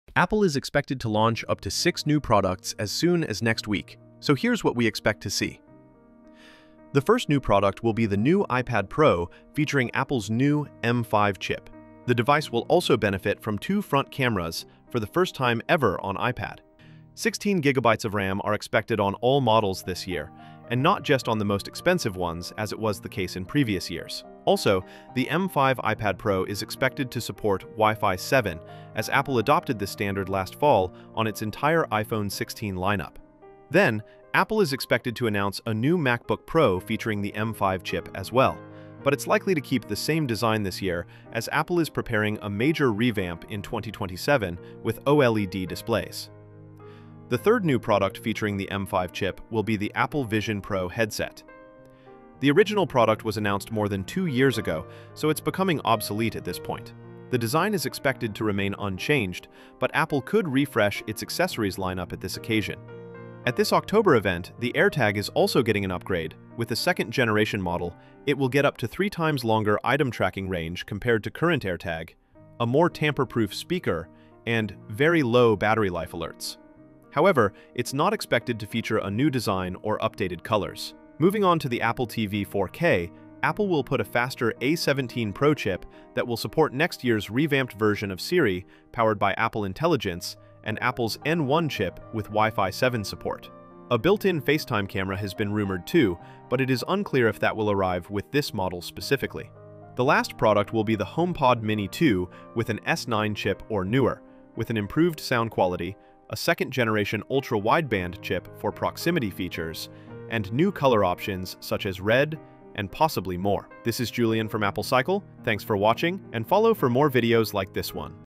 Apple October Event: 6 New Products Coming!
Apple is expected to release up to six new products next week! The M5 chip will be introduced on the iPad Pro, MacBook Pro and Vision Pro as Apple plans to upgrade its VR headset for the first time ever in two years. We also expect some minor upgrades on AirTag, Apple TV 4K and HomePod mini.

Chapters:

00:06 M5 iPad Pro
00:34 M5 MacBook Pro
00:47 M5 Vision Pro
01:03 AirTag 2
01:21 Apple TV 4K
01:39 HomePod mini 2